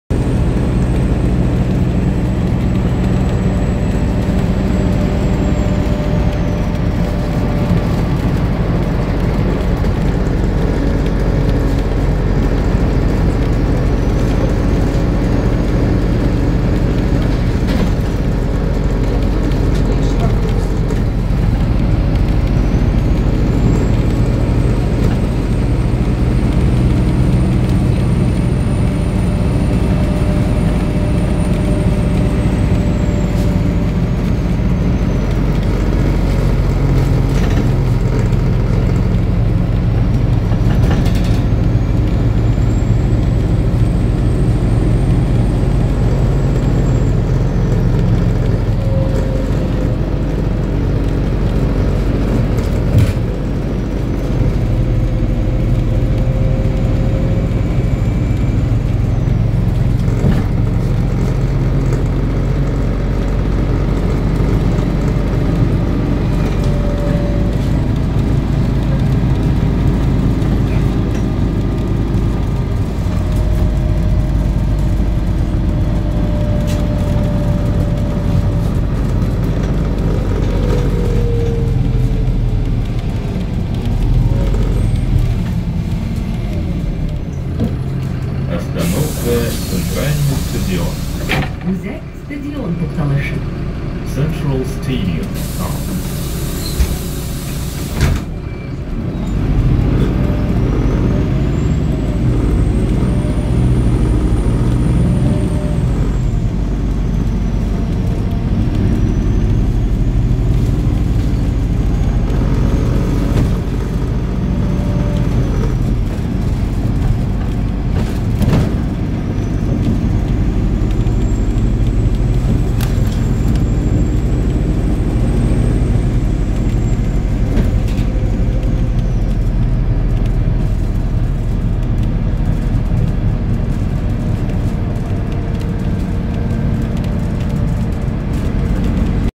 Казанский кремль в окне автобуса.
Республика Татарстан, город Казань. Казанский кремль в окне автобуса.